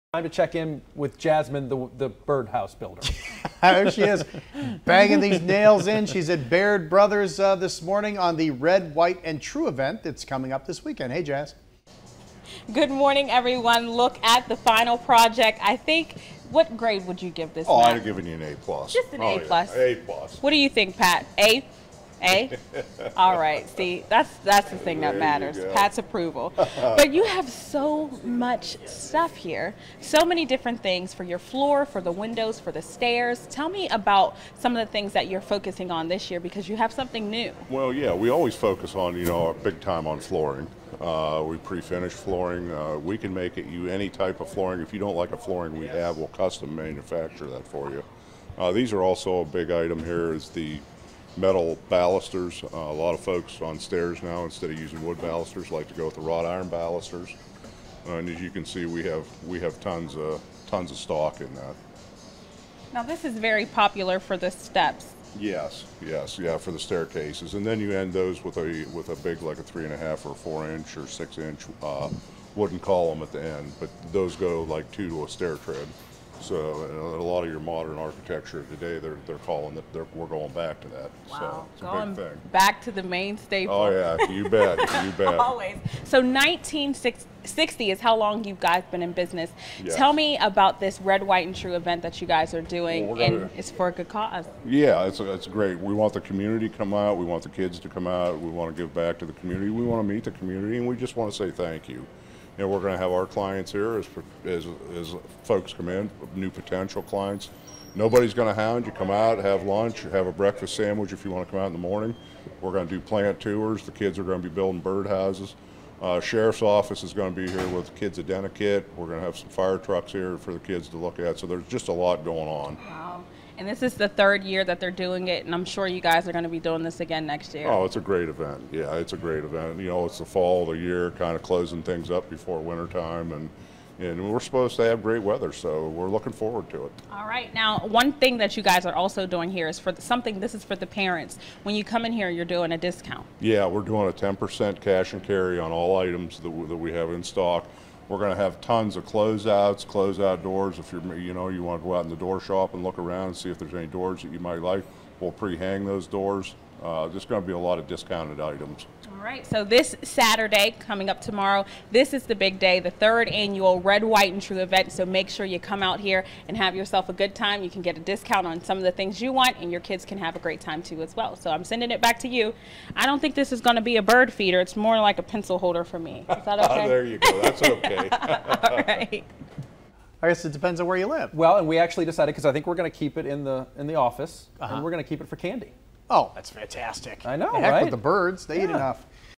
Red, White & True - 2014 | WFMJ Today Feature - Part 3
WFMJ Today feature, previewing what to expect at our 3rd Annual Red, White & True Celebration.

FOR THE PLACE YOU LOVE.